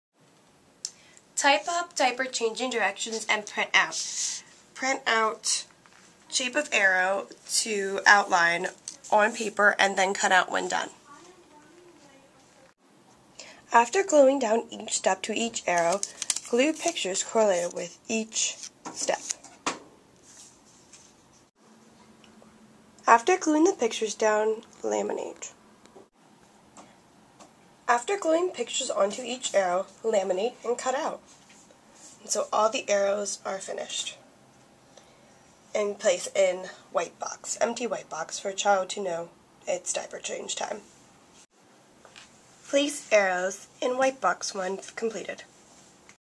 Diaper Changing Routine MAKE IT
Description: The Diaper Changing Routine visual support will help infants and toddlers become familiar with the routine and prepare for the diaper change. Each step of the diaper changing routine is displayed on an arrow  with a picture of the step so the child can see one step at a time and anticipate which step will be coming next. The child can hold each arrow and place it  in an empty wipe container when each step is completed.
Age appropriate: 6 months and older